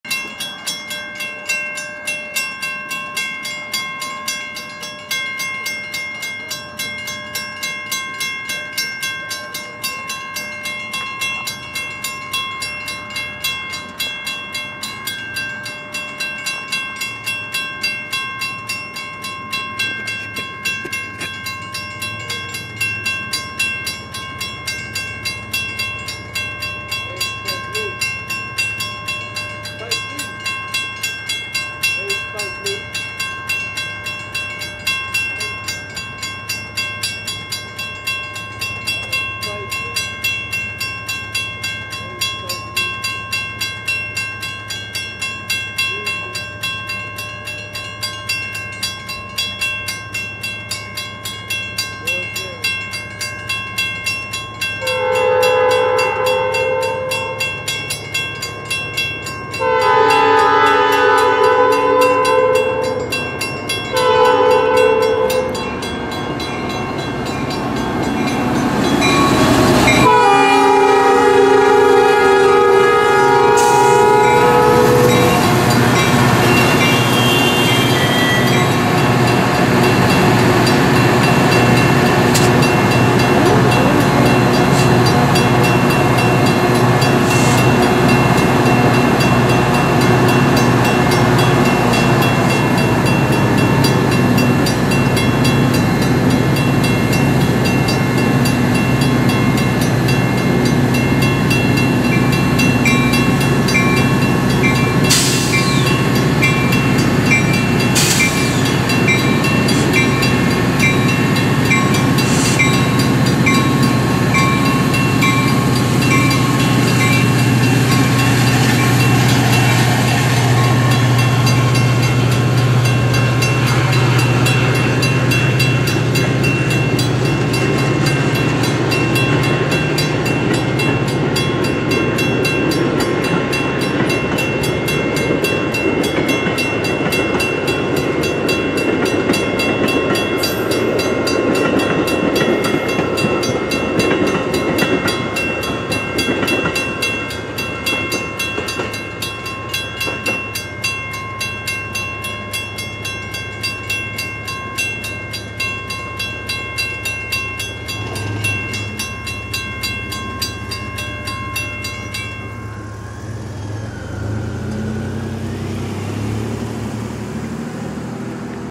Metra 202 West in Robbins, IL 10/29/19
WB Metra, 139th St., Robbins, IL, Metra Rock Island District
Metra 202 = F40PHM-3

Special thanks to my Patrons:
David Prince
Austin Burns
August Hug